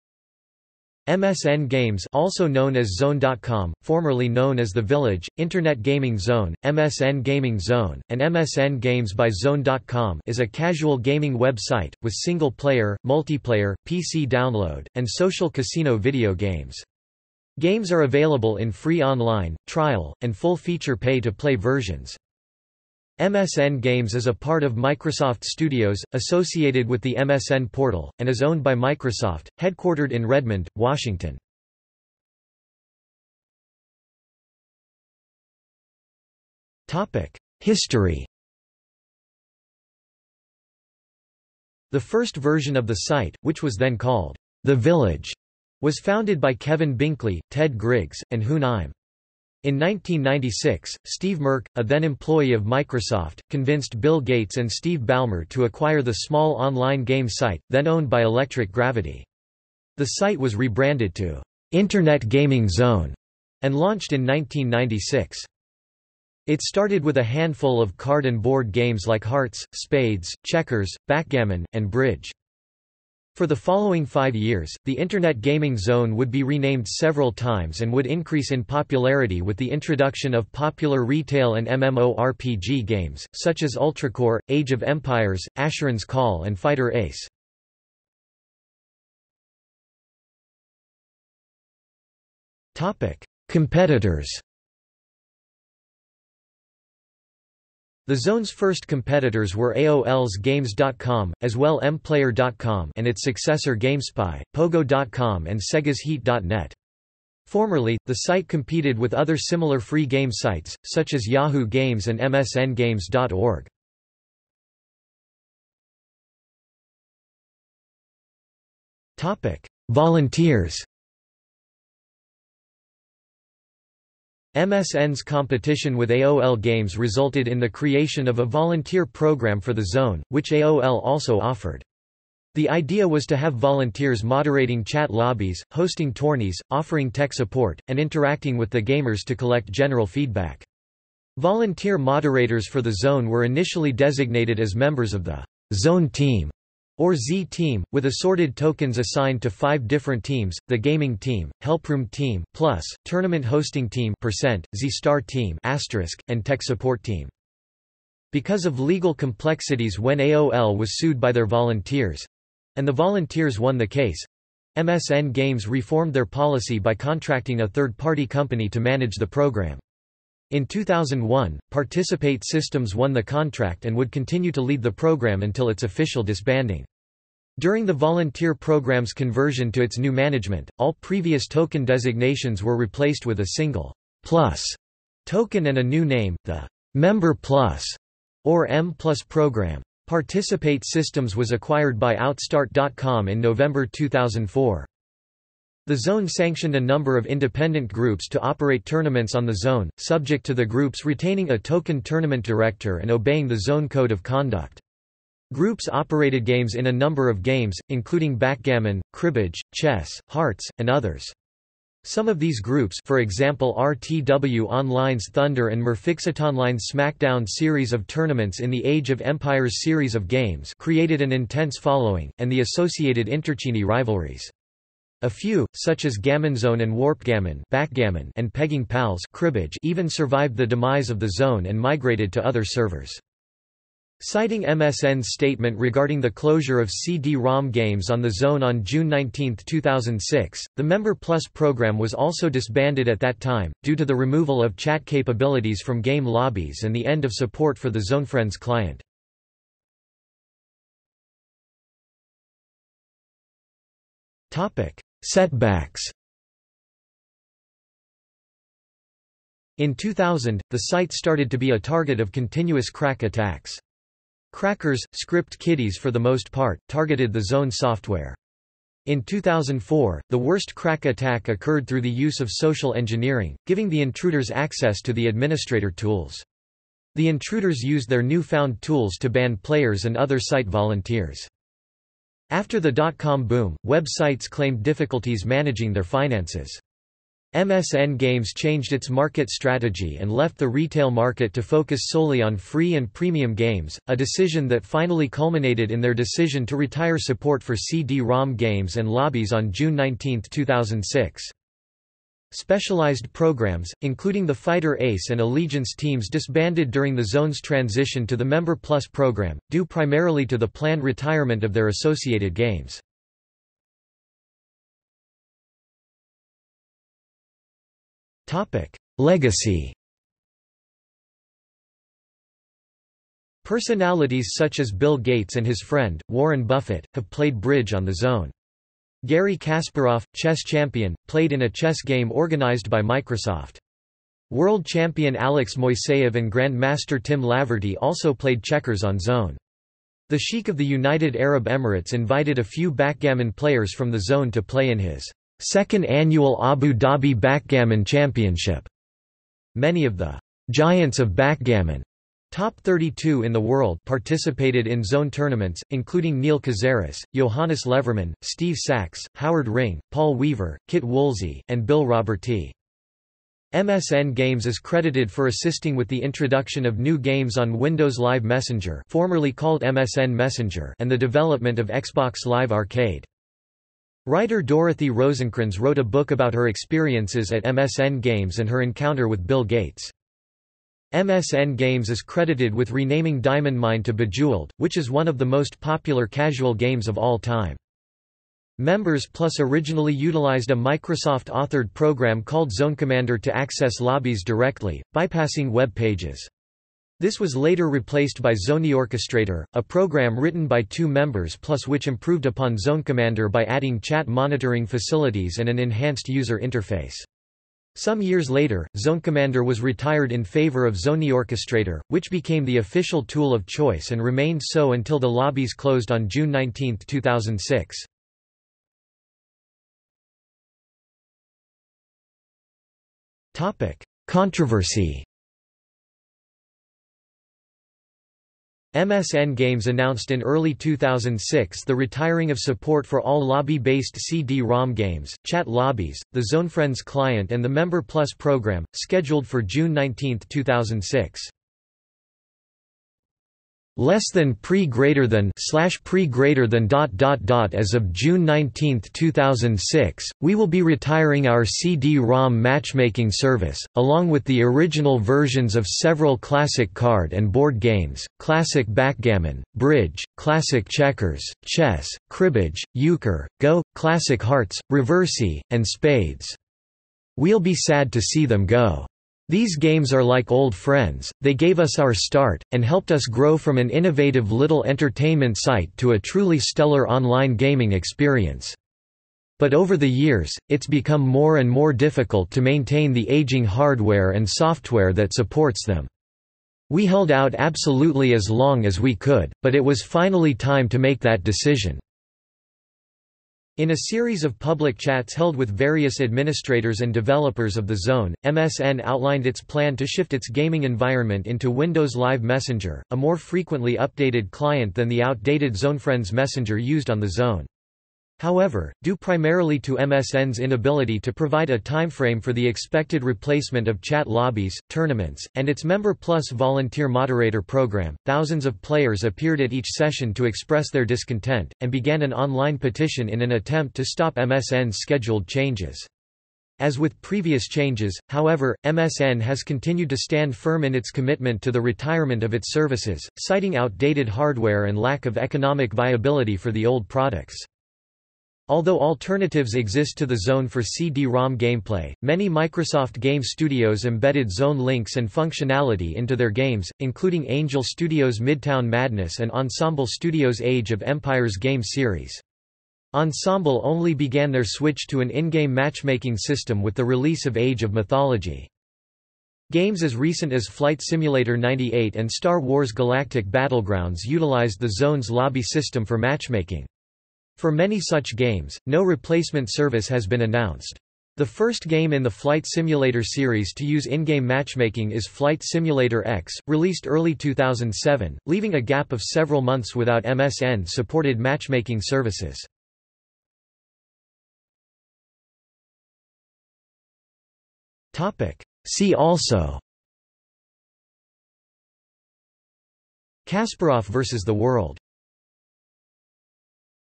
MSN Games | Wikipedia audio article
00:00:33 1 History
00:01:29 2 Competitors
00:01:55 3 Volunteers
00:04:10 4 Setbacks
00:05:14 5 Legacy
00:06:59 6 Controversy
00:09:44 7 See also

- Socrates

SUMMARY
MSN Games (also known as Zone.com - formerly known as The Village, Internet Gaming Zone, MSN Gaming Zone, and MSN Games by Zone.com) is a casual gaming web site, with single player, multiplayer, PC download, and social casino video games. Games are available in free online, trial, and full feature pay-to-play versions.
MSN Games is a part of Microsoft Studios, associated with the MSN portal, and is owned by Microsoft, headquartered in Redmond, Washington.